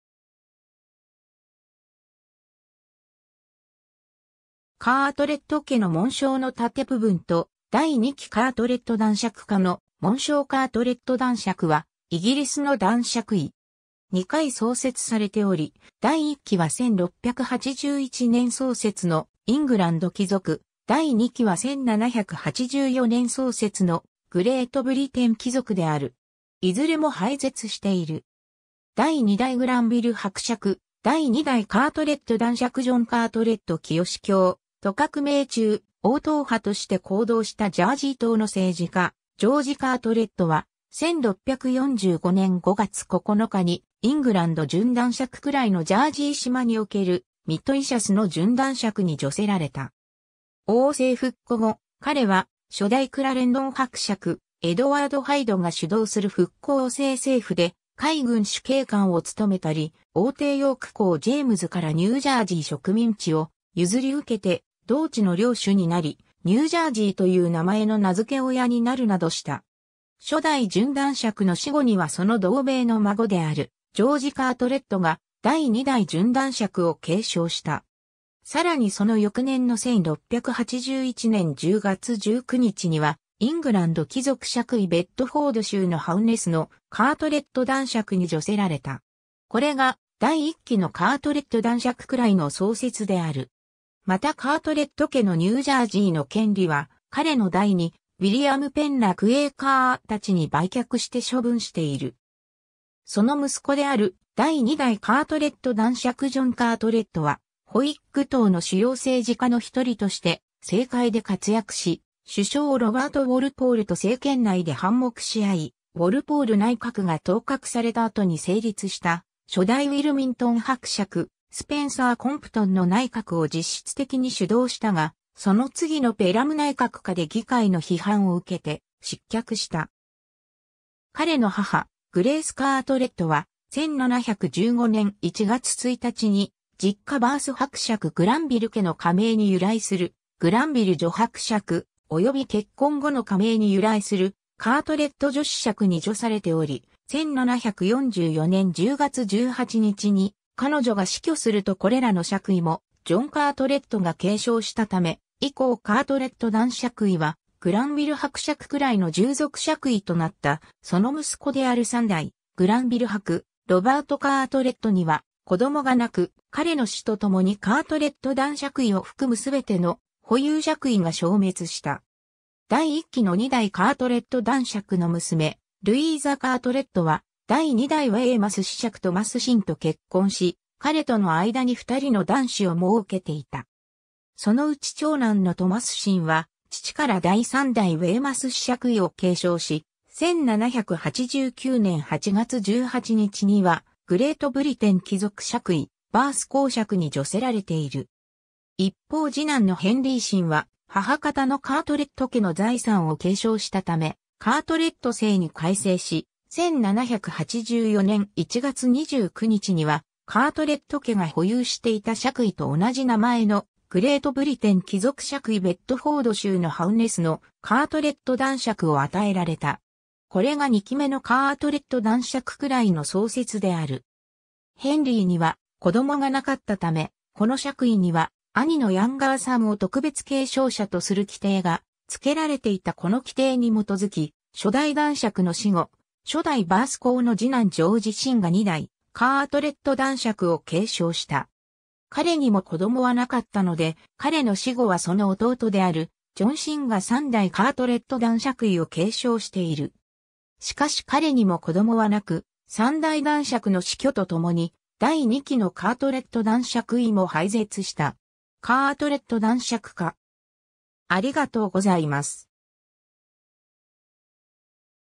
カートレット男爵
カートレット家の紋章の盾部分（左）と第2期カートレット男爵家の紋章（右） カートレット男爵（Baron Carteret）は、イギリスの男爵位。
2回創設されており、第1期は1681年創設のイングランド貴族、第2期は1784年創設のグレートブリテン貴族である。
いずれも廃絶している。
 第2代グランヴィル伯爵・第2代カートレット男爵ジョン・カートレット 清教徒革命中、王党派として行動したジャージー島の政治家ジョージ・カートレット(1610頃–1680)は、1645年5月9日にイングランド準男爵位の"ジャージー島におけるMeteschesの"準男爵（Baronet "of Metesches, in Jersey"）に叙せられた。
王政復古後、彼は初代クラレンドン伯爵エドワード・ハイドが主導する復古王政政府で海軍主計官を務めたり、王弟ヨーク公ジェームズ（ジェームズ2世）からニュージャージー植民地を譲り受けて同地の領主になり、ニュージャージーという名前の名付け親になるなどした。
 初代準男爵の死後にはその同名の孫であるジョージ・カートレット(1669–1695)が第2代準男爵を継承した。
さらにその翌年の1681年10月19日にはイングランド貴族爵位ベッドフォード州のハウネスのカートレット男爵(Baron Carteret of Hawnes, in the County of Bedford)に叙せられた。
これが第1期のカートレット男爵位の創設である。
またカートレット家のニュージャージーの権利は彼の代にウィリアム・ペンらクエーカーたちに売却して処分している。
 その息子である第2代カートレット男爵ジョン・カートレット(1690–1763)は、ホイッグ党の主要政治家の一人として政界で活躍し、首相ロバート・ウォルポールと政権内で反目し合い、ウォルポール内閣が倒閣された後に成立した初代ウィルミントン伯爵スペンサー・コンプトンの内閣を実質的に主導したが、その次のペラム内閣下で議会の批判を受けて失脚した。
彼の母グレイス・カートレット(1654–1744)は、1715年1月1日に実家バース伯爵グランヴィル家の家名に由来するグランヴィル女伯爵(Countess Granville)、および結婚後の家名に由来するカートレット女子爵(Viscountess Carteret)に叙されており（いずれもグレートブリテン貴族爵位）、1744年10月18日に彼女が死去するとこれらの爵位もジョン・カートレットが継承したため、以降カートレット男爵位はグランヴィル伯爵位の従属爵位となった その息子である3代グランヴィル伯(3代カートレット男爵)ロバート・カートレット(1721–1776)には子供がなく、彼の死とともにカートレット男爵位を含むすべての保有爵位が消滅した。
 第1期の2代カートレット男爵(2代グランヴィル伯)の娘ルイーザ・カートレットは、第2代ウェイマス子爵トマス・シンと結婚し、彼との間に2人の男子を儲けていた。
そのうち長男のトマス・シン(1734年-1796年)は、父から第3代ウェイマス子爵位を継承し、1789年8月18日にはグレートブリテン貴族爵位バース侯爵に叙せられている。
一方次男のヘンリー・シン(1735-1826)は、母方のカートレット家の財産を継承したため、カートレット姓に改姓し、1784年1月29日にはカートレット家が保有していた爵位と同じ名前のグレートブリテン貴族爵位ベッドフォード州のハウネスのカートレット男爵(Baron Carteret of Hawnes, in the County of Bedford)を与えられた。
これが2期目のカートレット男爵位の創設である。
ヘンリーには子供がなかったため、この爵位には兄(初代バース侯)のヤンガーサン(次男以下)を特別継承者(special remainder)とする規定が付けられていた この規定に基づき、初代男爵の死後、初代バース侯の次男ジョージ・シン(1770–1838)が2代カートレット男爵を継承した。
彼にも子供はなかったので、彼の死後はその弟(初代バース侯の三男)であるジョン・シン(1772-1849)が3代カートレット男爵位を継承している。
しかし彼にも子供はなく、3代男爵の死去とともに第2期のカートレット男爵位も廃絶した。
 カートレット男爵家